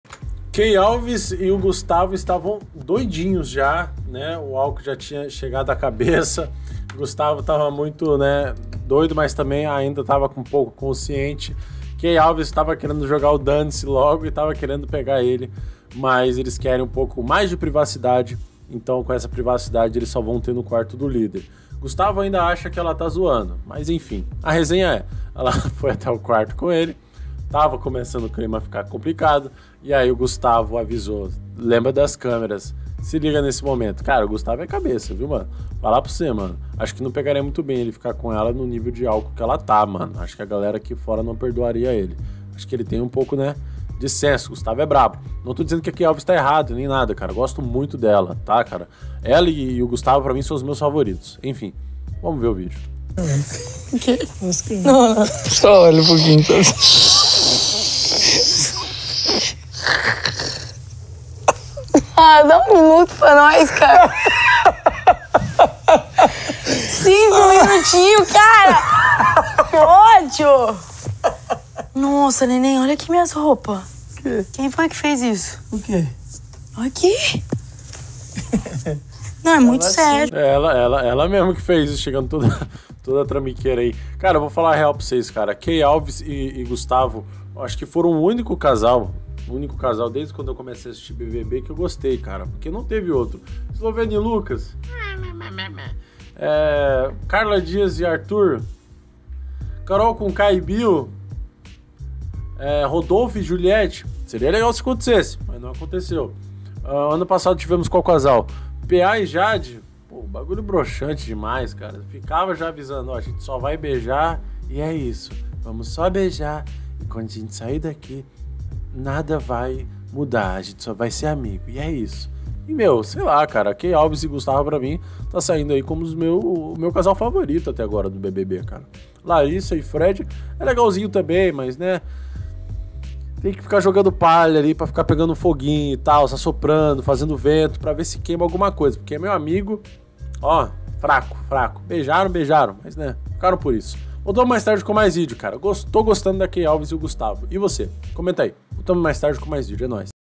ISSO CORTOU O CLIMA!!! KEY ALVES IA COMEÇAR A FAZER COM GUSTAVO MAS ELE AVISA ELA DAS CAMERAS
Compre o PRODUTO agora mesmo! e aproveite.